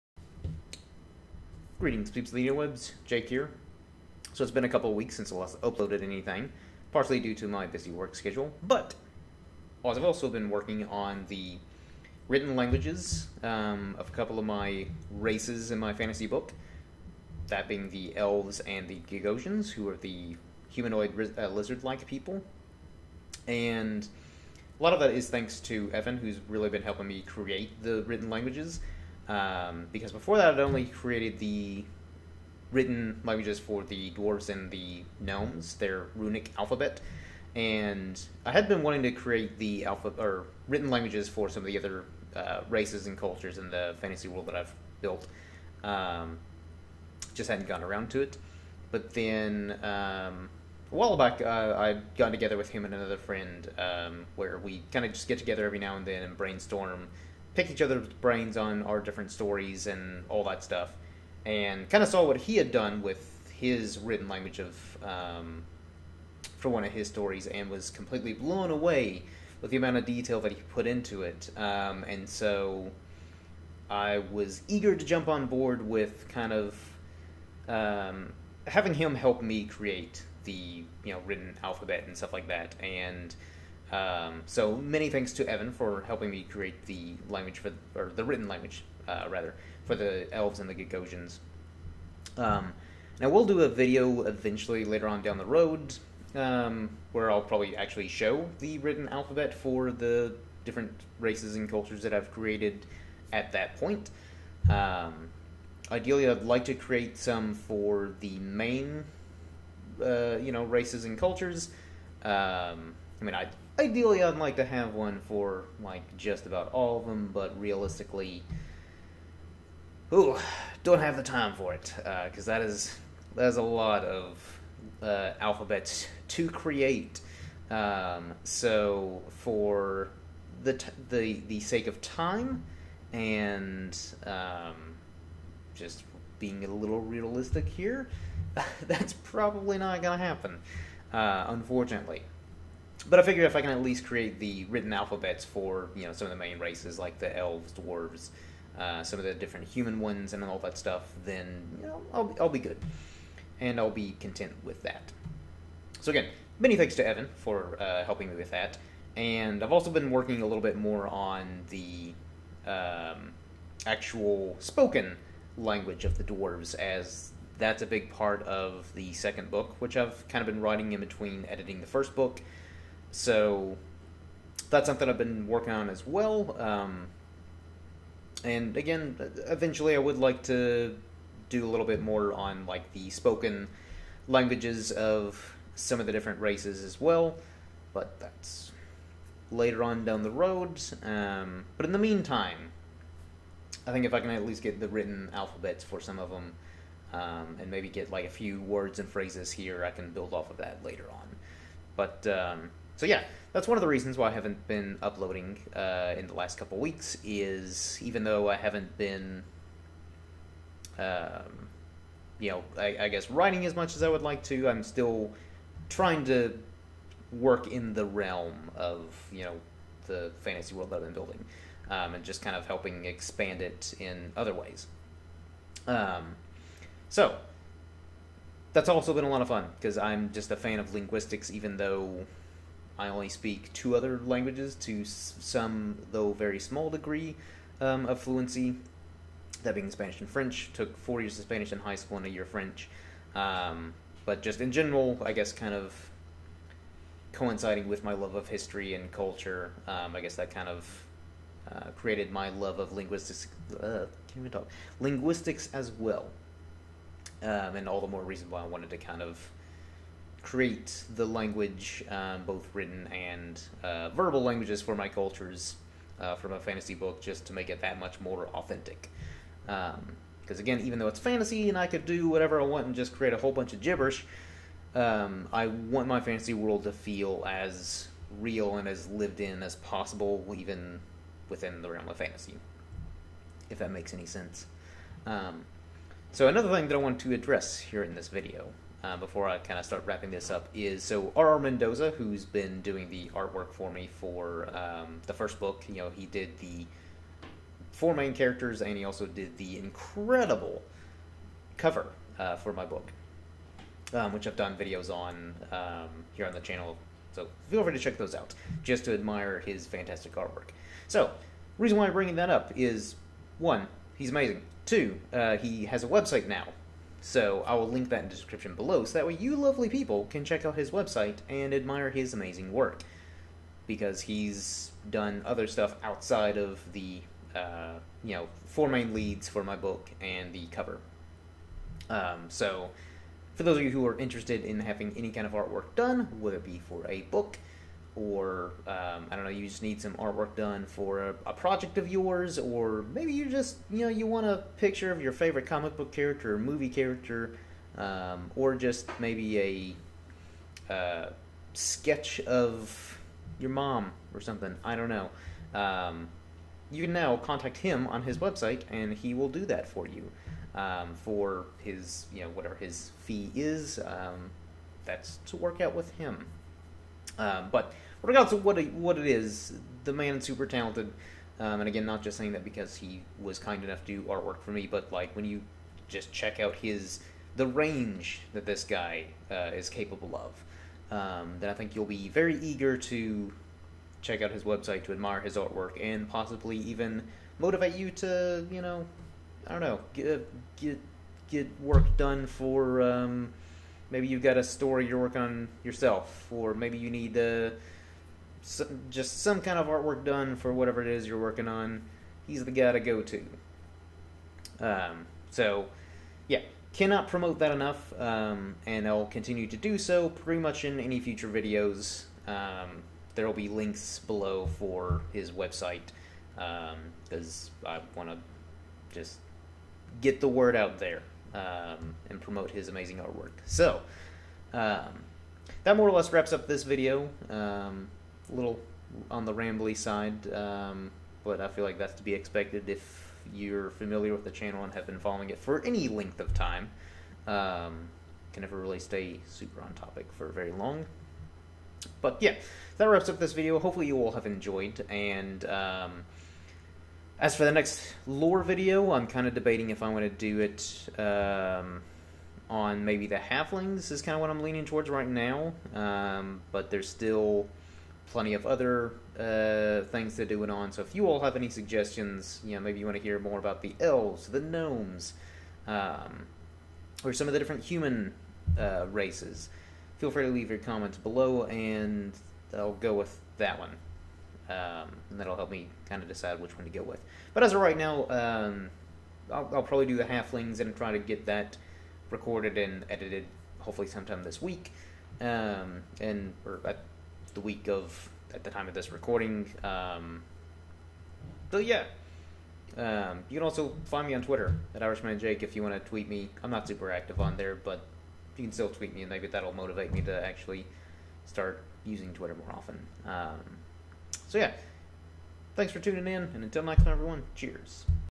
Creating Languages for My Book Series
In this video I talk a little bit about how I've been creating some of the languages for the different races and cultures within my fantasy book. I also talk about the recently launched website of R.R. Mendoza who's done the amazing artwork for my book which you can check out So do me and him and favor by checking out his incredible artwork!